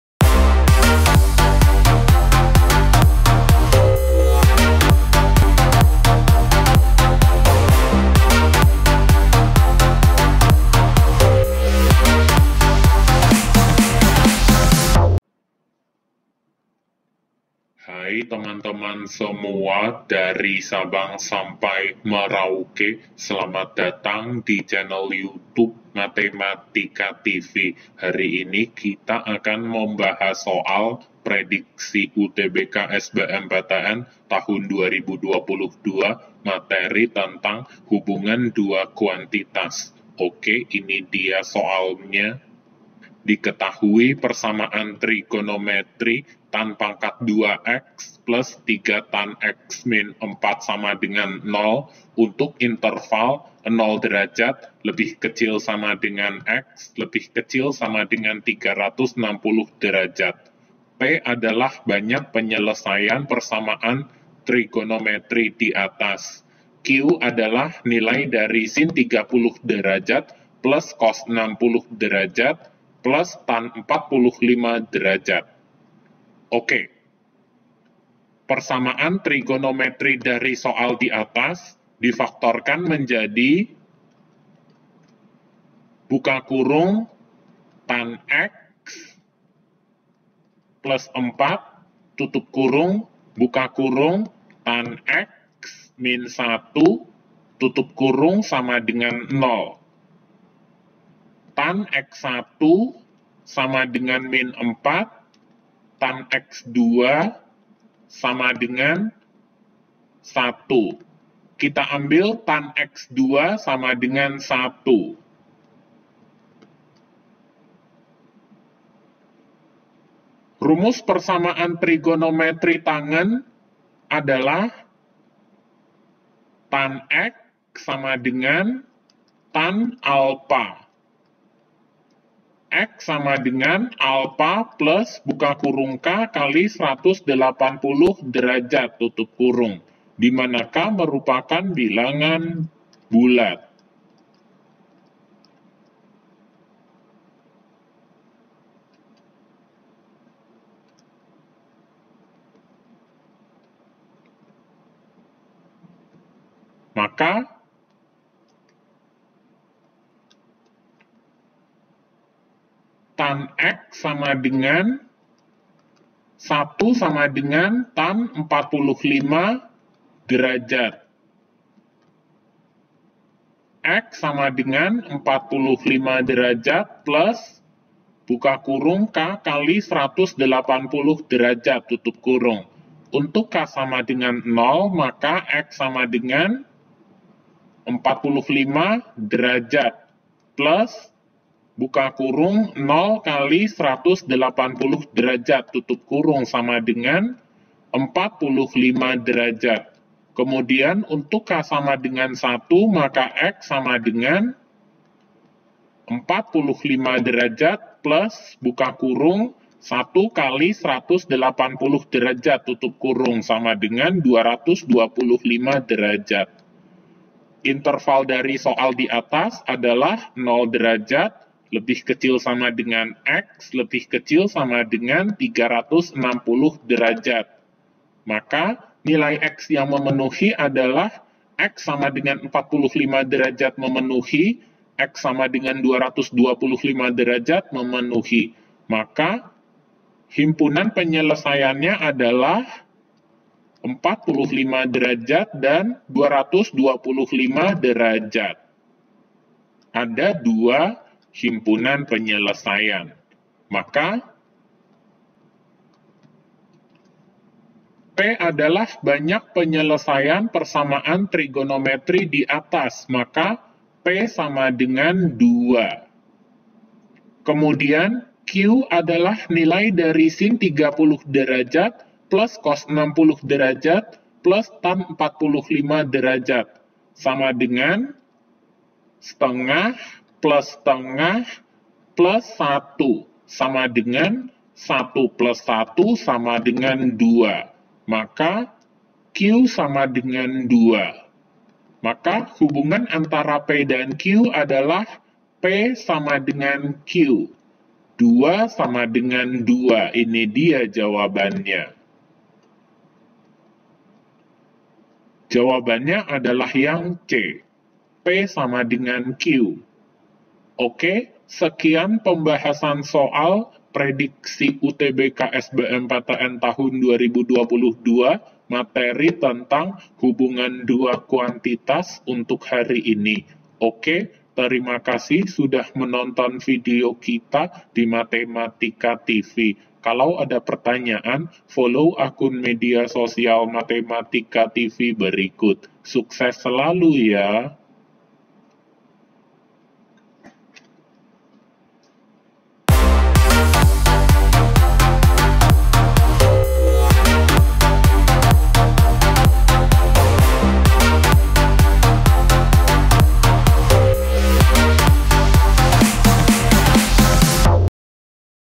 [MATEMATIKA TV] PEMBAHASAN SOAL PREDIKSI UTBK SBMPTN 2022: HUBUNGAN DUA KUANTITAS (005)
Pada kesempatan kali ini kita akan membahas soal prediksi UTBK SBMPTN tahun 2022 materi tentang hubungan dua kuantitas: persamaan trigonometri.